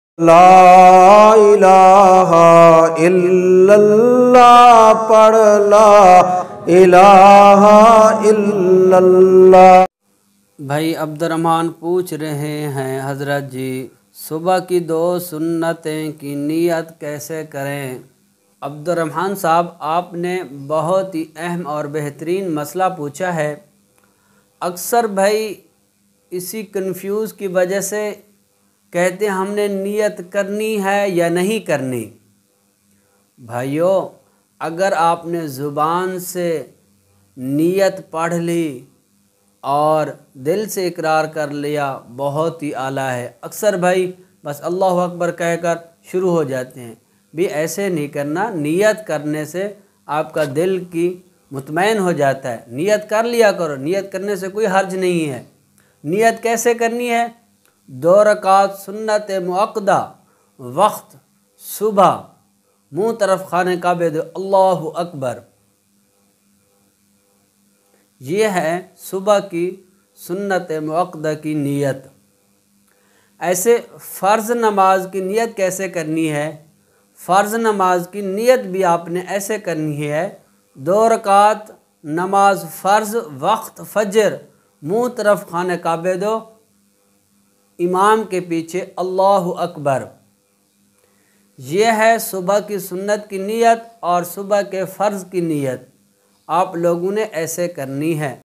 fajar ki sunnat ki niyat kaise kare ? | Sheikh ul wazaif Hazrat Maulana Zafar iqbal Nafees
Fajar Ki Namaz Ki Niyat Fajar Ki Namaz Ki Niyat Kaise Karte Hain | Namaz Ki Niyat Ka Tarika
Maulana Zafar iqbal Nafees